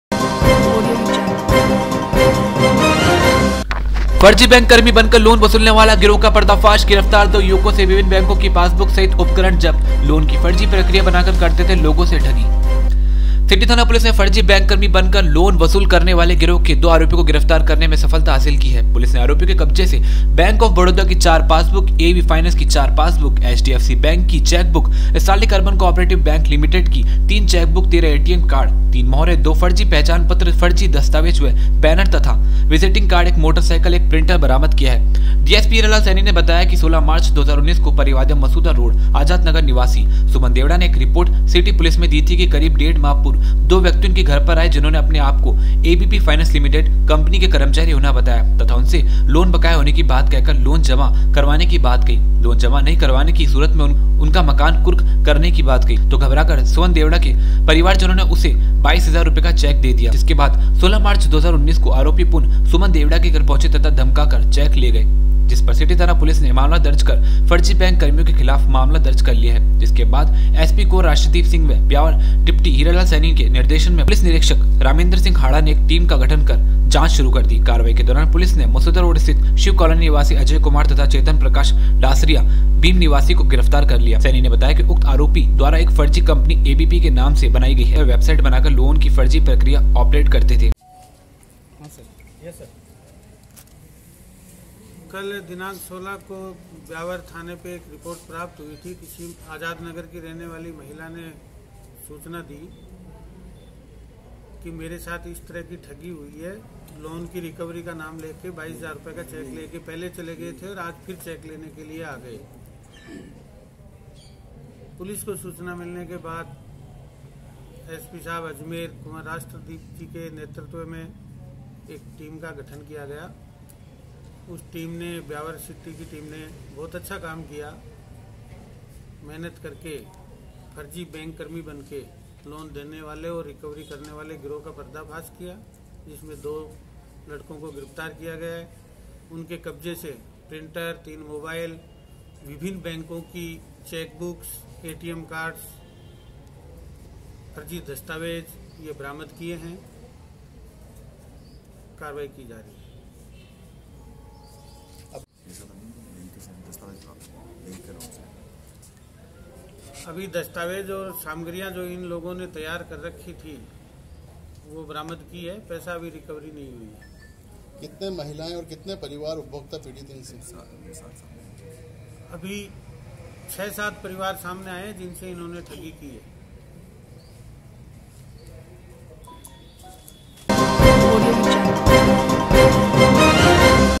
HORIZON HIND NEWS - फर्जी बैंक कर्मी बनकर लोन वसूलने वाले गिरोह का पर्दाफाश
फर्जी बैंक कर्मी बनकर लोन वसूलने वाले गिरोह का पर्दाफाश
गिरफ्तार 2 युवको से विभिन्न बेंको की पासबुके सहित उपकरण जब्त
लोन की फर्जी प्रक्रिया बनाकर करते थे लोगो से ठगी
ब्यावर, (हेमन्त साहू)। सिटी थाना पुलिस ने फर्जी बैंककर्मी बनकर लोन वसूल करने वाले गिरोह के दो आरोपियों को गिरफतार करने मे सफलात हासिल की है। पुलिस ने आरोपियो के कब्जे से बैंक आॅफ बडोदा की चार पास बुक, एवी फाईनेंस की चार पास बुक, एचडीएफसी बैंक की चेक बुक, स्टर्लिक अरबन काॅआपरेटिव लिमिटेड की तीन चेक बुक 13 ऐटीएम कार्ड तीन मोहरे, दो फर्जी पहचान पत्र, फर्जी दस्तावेज व बेनर तथा विजिटिंग कार्ड, एक मोटर साईकिल, एक प्रिंटर बरामद किए है। डीएसपी  हीरालाल सेनी ने बताया कि 16 मार्च 2019 को पिरवादिया मसूदा रोड आजाद नगर निवासी श्रीमति सुमन देवडा ने एक रिर्पोट सिटी पुलिस मे दी थी कि करीब डेढ माह पूर्व दो व्यक्ति उनके घर पर आए जिन्होने अपने आप को एपीबी फाइनेंस लिमिटेड कम्पनी के कर्मचारी होना बताया तथा उनसे लोन बकाया होने की बात कहकर लोन जमा करवाने की बात कही लोन जमा नही करवाने की सूरत मे उनका मकान कूर्क करने की बात कही। तो घबराकर सुमन देवडा के परिवारजनो ने उसे 22 हजार रूपये का चेक दे दिया।
जिसके बाद 16 मार्च 2019 को आरोपी पुन सुमन देवडा के घर पहुंचे तथा धमका कर चेक ले गये। जिस पर सिटी पुलिस ने मामला दर्ज कर फर्जी बेंक कर्मियो के खिलाफ मामला दर्ज कर लिया गया। जिसके बाद एसपी कुंवर राष्ट्रदीप सिंह व ब्यावर डिप्टी हीरालाल सेनी के निर्देशन मे पुलिस निरिक्षक रमेंद्रसिंह हाडा ने एक टीम का गठन कर जांच शुरू कर दी। कार्यवाही के दौरान पुलिस ने मसूदा रोड स्थित शिव कोलोनी निवासी अजय कुमार तथा चेतन प्रकाश डासरिया भीम निवासी को गिरफतार कर लिया। सैनी ने बताया कि उक्त आरोपी द्वारा एक फर्जी कम्पनी ऐपीबी के नाम से बनाई गई आरे वेबसाइट बनाकर लोन की फर्जी प्रक्रिया आॅपरेट करते थे। आरोपियों से शहर में हुई अन्य वारदातों का खुलासा होने की संभावना है। गटित टीम में हैड कांस्टेबल केलाशचंद, जितेंद्रसिंह, भगवानसिंह, भवानीसिंह, राजाराम, कृष्ण कुमार, विजय तथा रोहिताश्व शामिल थे।